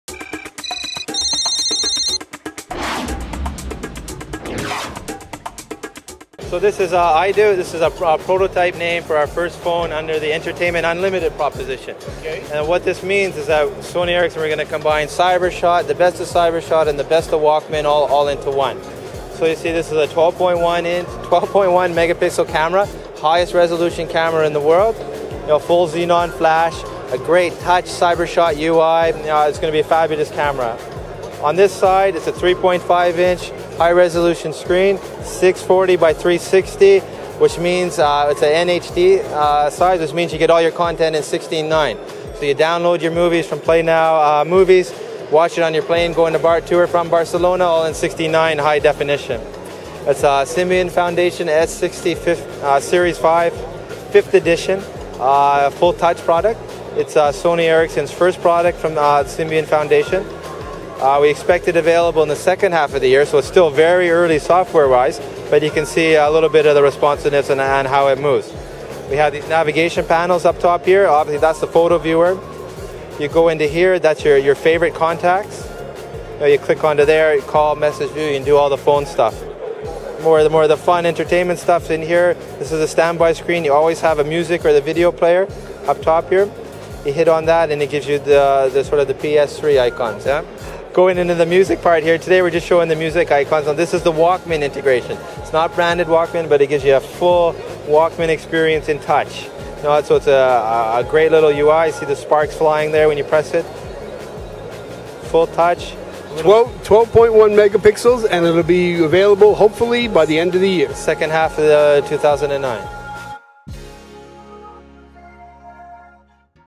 Sony Ericsson First Look Mobile World Congress 2009
Sony Ericsson is working on a touchscreen phone with a 12.1-megapixel camera